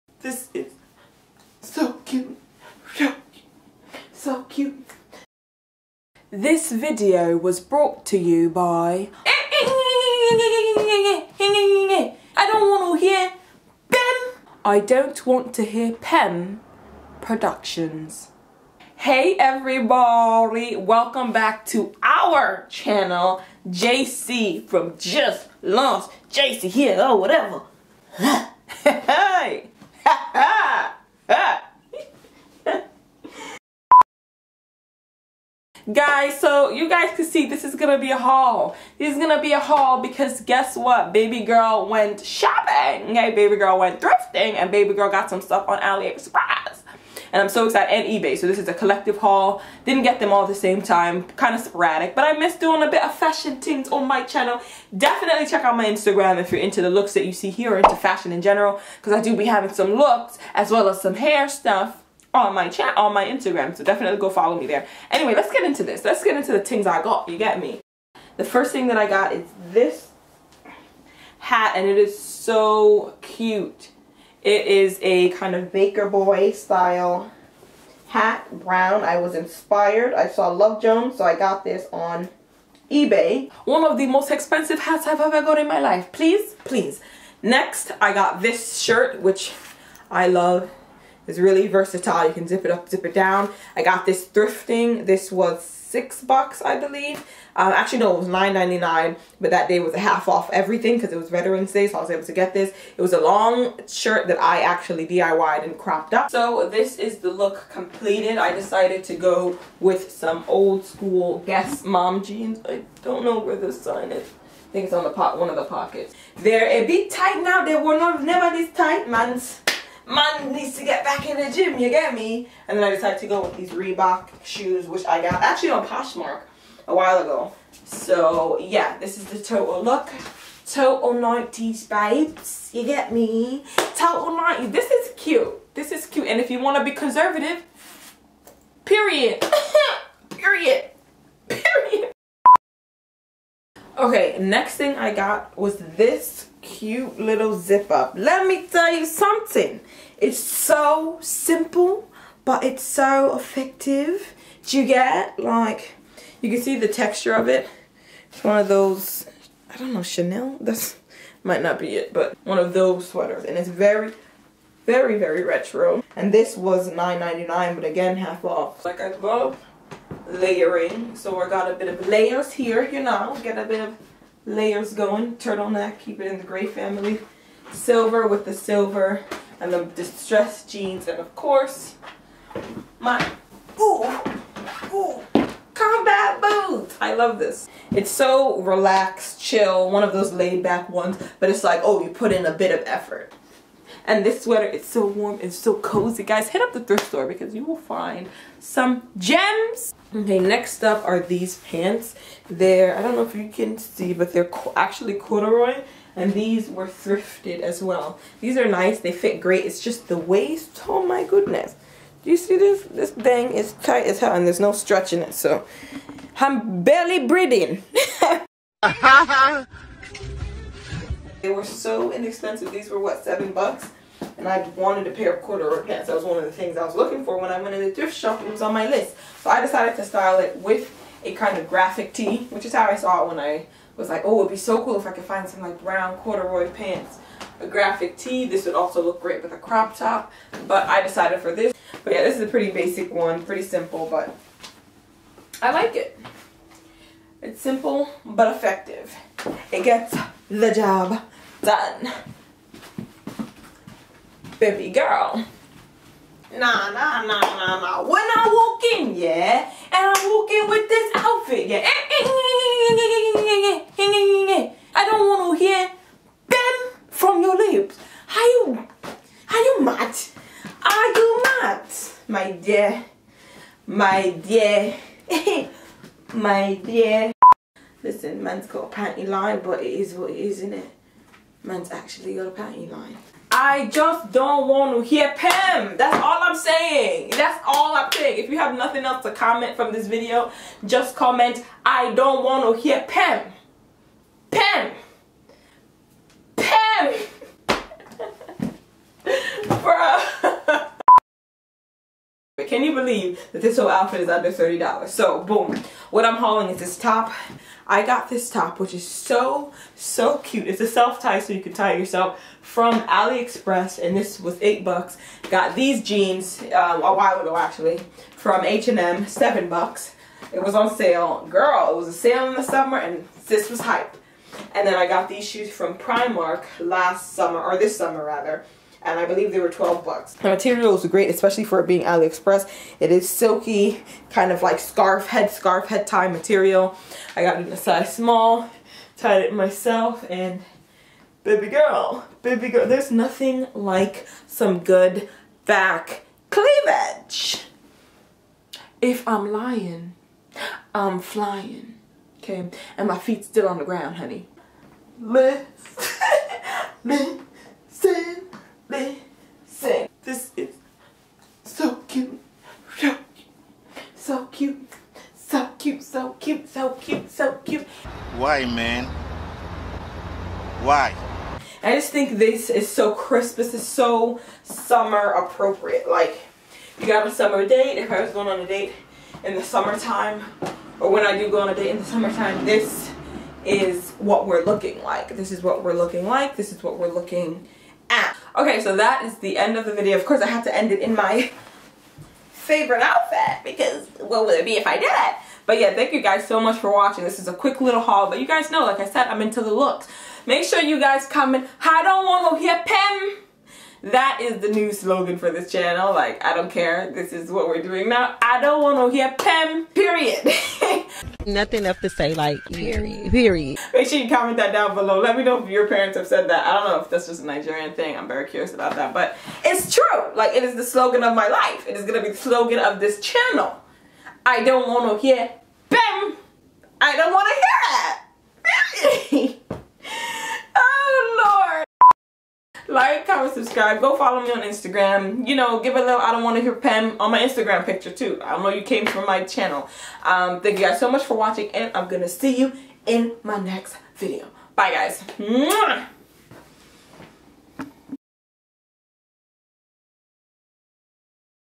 little booties matter: collective thrift, aliexpress, ebay haul
Some of my recent purchases and how I style them. Yall I felt cute and was acting up lol

aliexpress top:

Editor
Sony Vegas Pro 11
Camera